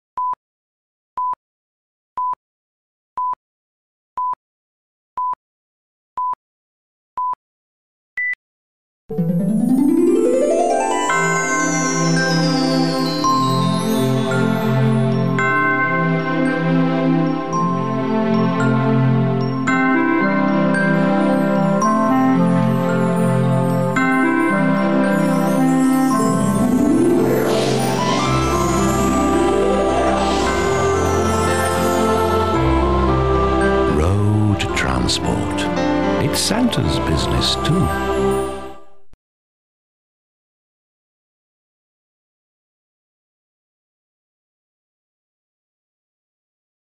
Santa's Best Friend
CNN commercial: Road transport is Santa's business too...